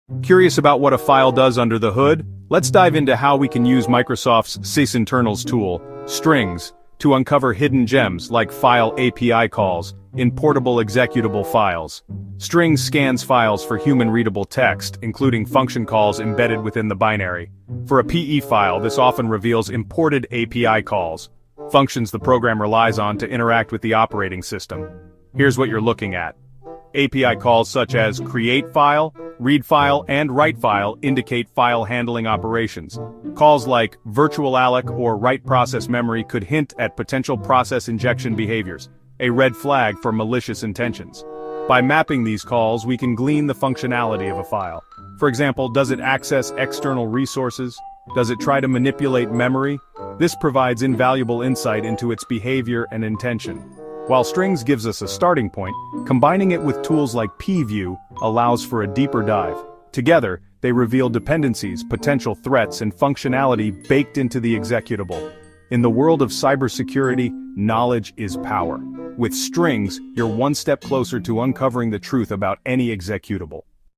Uncover Hidden Secrets in PE Files with strings short
Unlock the mysteries of Portable Executable (PE) files with Microsoft's powerful Sysinternals tool, "Strings"! In this insightful video, we demonstrate how to analyze files for hidden API calls and understand their behavior. Discover how Strings reveals critical functions like CreateFile and WriteProcessMemory, providing vital clues about potential malicious activity.

Join us as we explore how to map these API calls to gain insights into file functionality and uncover hidden threats. We'll also show you how to enhance your analysis by combining Strings with other tools like PEview for a comprehensive examination.